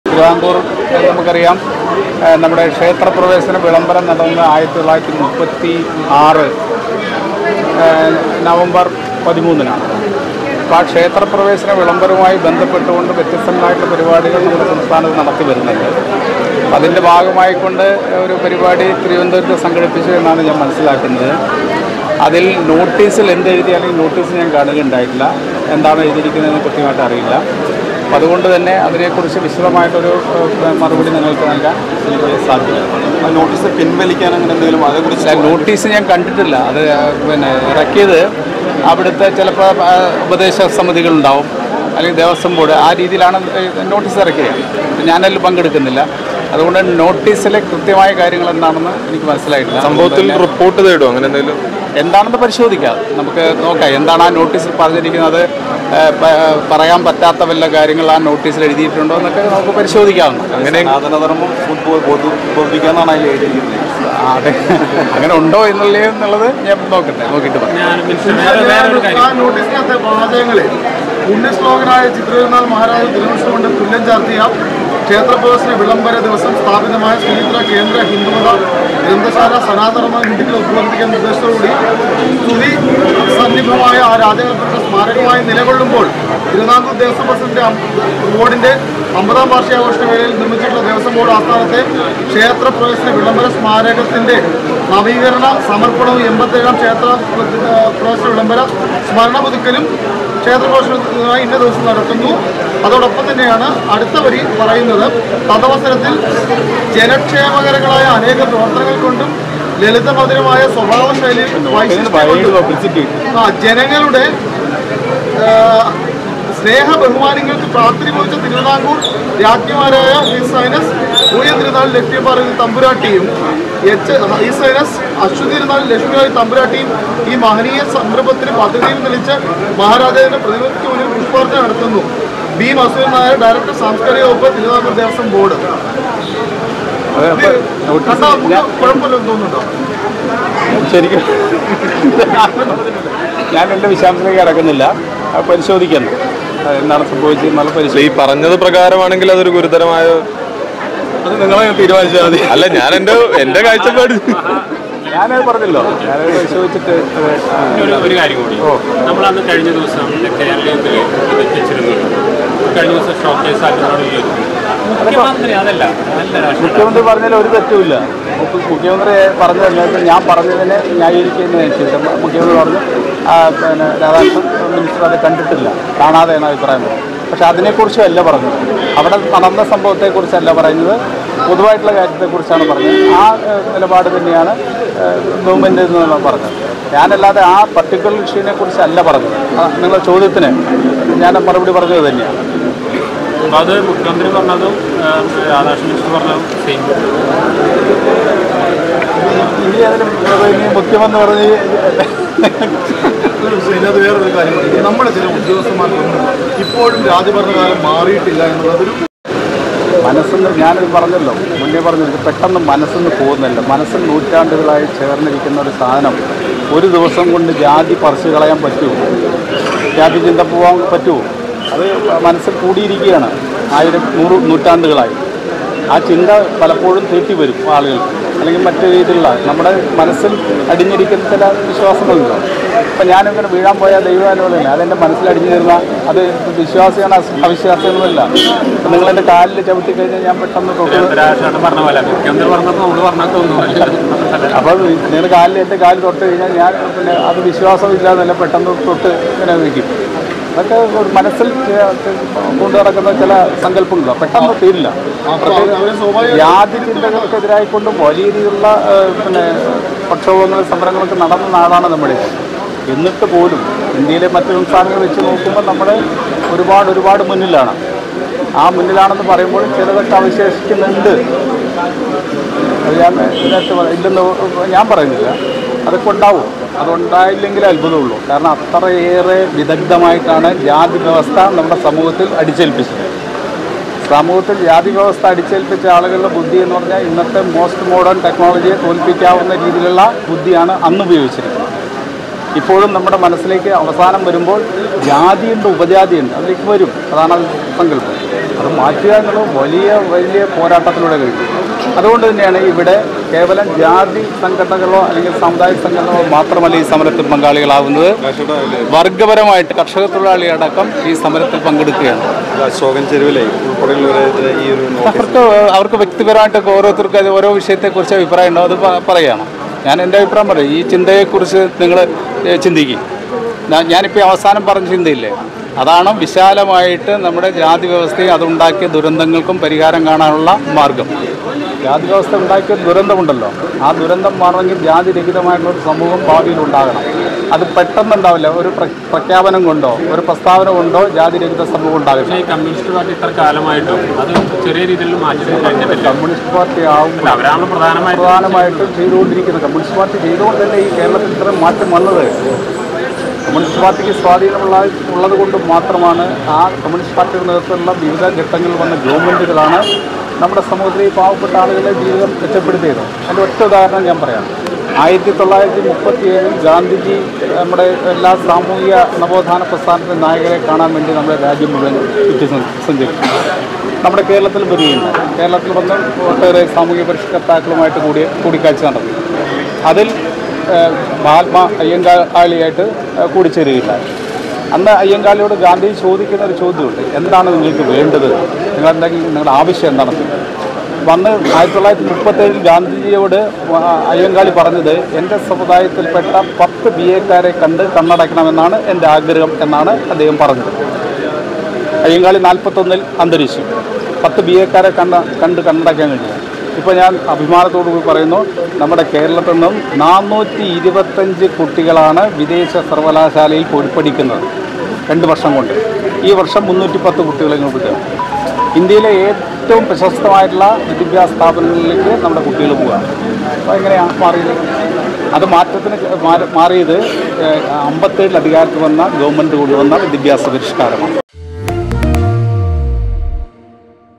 നോട്ടീസിലെ വിവാദം പരിശോധിക്കും മന്ത്രി.കെ.രാധാകൃഷ്ണന്‍
ക്ഷേത്രപ്രവേശന 87-ാം വാര്‍ഷികത്തോട് അനുബന്ധിച്ച് തിരുവിതാംകൂര്‍ദേവസ്വം ബോര്‍ഡ് പുറത്തിറക്കിയ നോട്ടിസിലുയര്‍ന്ന വിവാദം പരിശോധിക്കുമെന്ന് ദേവസ്വംമന്ത്രി കെ.രാധാകൃഷ്ണന്‍. മനസില്‍ അടിഞ്ഞിരിക്കുന്ന ജാതിചിന്ത പെട്ടെന്ന് പോകില്ല. മനസില്‍ നൂറ്റാണ്ടുകളായി ചേര്‍ന്നിരിക്കുന്ന ജാതി ചിന്ത ഒരുദിവസം കൊണ്ട് പറിച്ചുകളയാന്‍ പറ്റുമോ? ആ ചിന്ത ആളുകള്‍ക്ക് പലപ്പോഴും തികട്ടിവരും. സമൂഹത്തില്‍ ജാതി വ്യവസ്ഥ അടിച്ചേല്‍പ്പിച്ച ആളുകളുടെ ബുദ്ധി ഇന്നത്തെ മോസ്റ്റ് മോഡേണ്‍ ടെക്നോളജിയെ തോല്‍പ്പിക്കാനാകുന്ന വിധത്തിലുള്ള ബുദ്ധിയാണ് അന്ന് ഉപയോഗിച്ചിരുന്നതെന്നും നോട്ടിസ് പരിശോധിക്കുമെന്നും മന്ത്രി തൃശൂരില്‍ മാധ്യമങ്ങളോട് പറഞ്ഞു